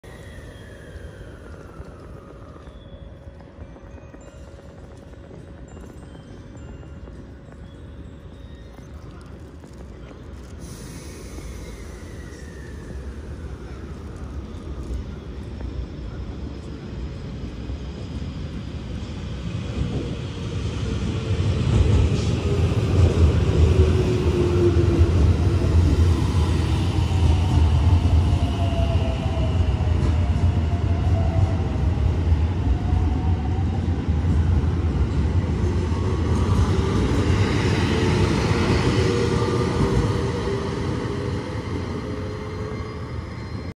223系＋5000系快速マリンライナー16号岡山行き到着と快速マリンライナー15号高松行き発車！ 瀬戸大橋線児島駅
JR四国とJR西日本の境界駅の瀬戸大橋線児島駅り、2番のりばに到着するJR西日本223系500番台＋5000系5両編成。9時25分発快速マリンライナー16号岡山行きと3番のりばか発車する9時23分発快速マリンライナー15号高松行き。